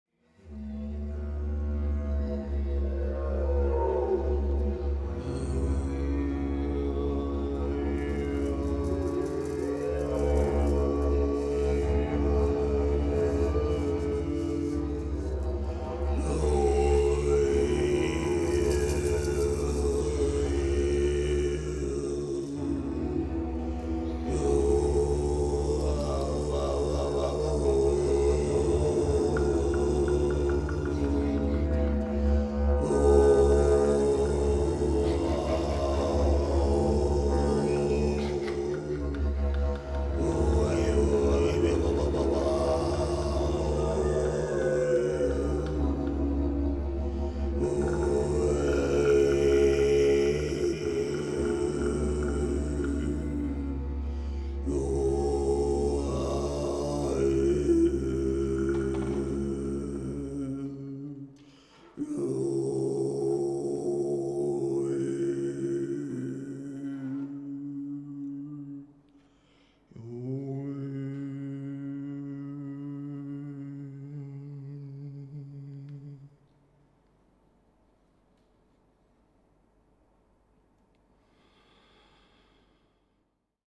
Torokhangokat gyakorlok
Ezek a speciális torokhangok alacsony frekvenciákat bocsátanak ki, amely a belső szerveket is teljesen átrezegtetik. Gyógyító hangoknak is hívják őket. Nekem közel 2 éves gyakorlás után jött meg ez a képesség. Mély tudati szintre is hatással van, módosult tudatállapotot hoz létre mind bennem, mind a hallgatóságban. Mikor meditációkat vezetek, bizonyos esetekben ezek segítenek, hogy a belső erőinkkel, illetve magasabb szellemi vezetőkkel kapcsolatba lépjünk. Hallgatása közben tudati mélységekbe és a szellemi régiókba is emelkedünk. Ajánlom mindenkinek! Daróczi Zoltán 06/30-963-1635